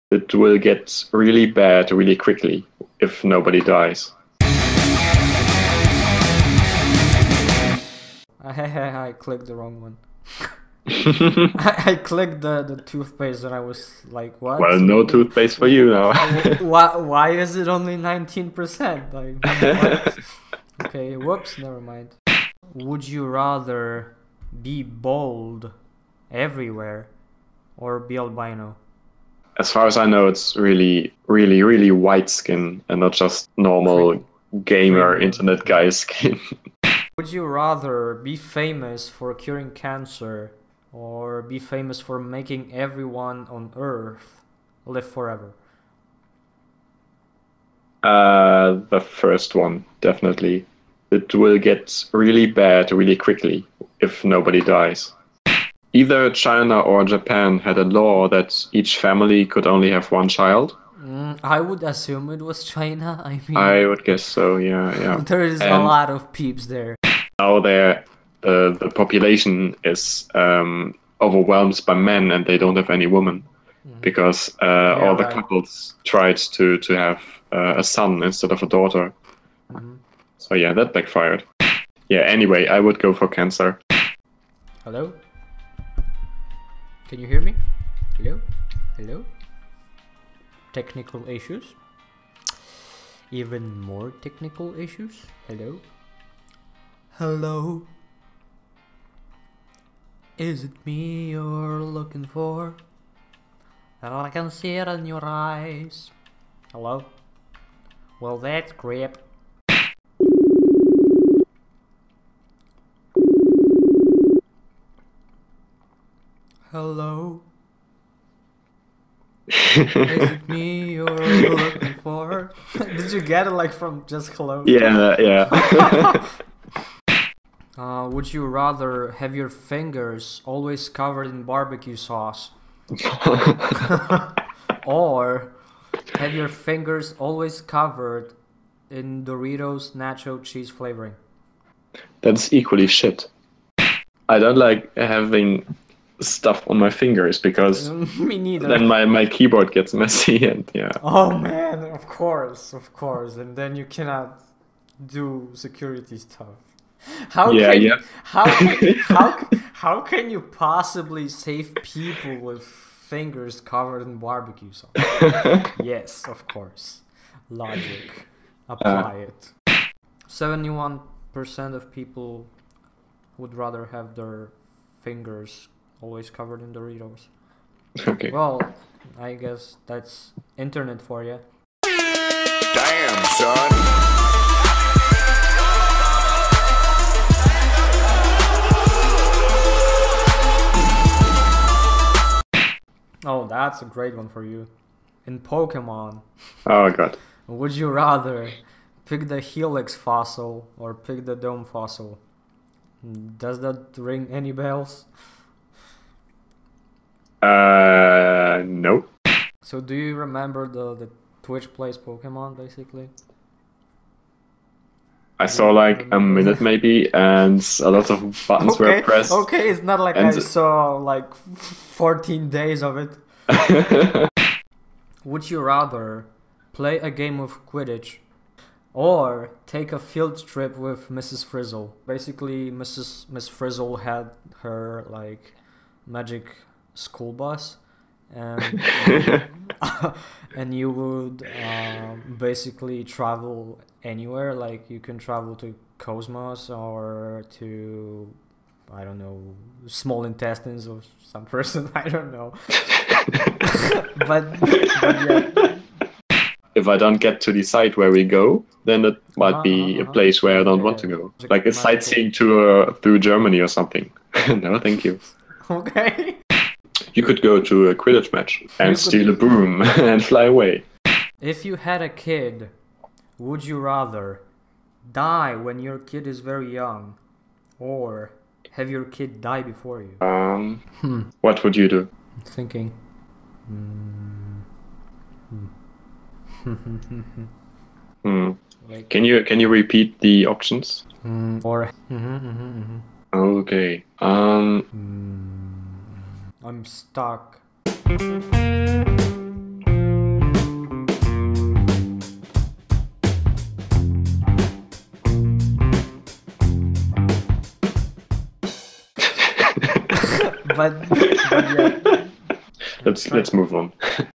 Would You Rather w/ 25A0 | Funny Moments
Some time ago together with Moritz @25A0 we played "Would You Rather". This is what came out of that :)
Jakiś czas temu razem z Moritzem @25A0 zagraliśmy w "Would You Rather". Pomyślałem, że coś z tego zrobię :)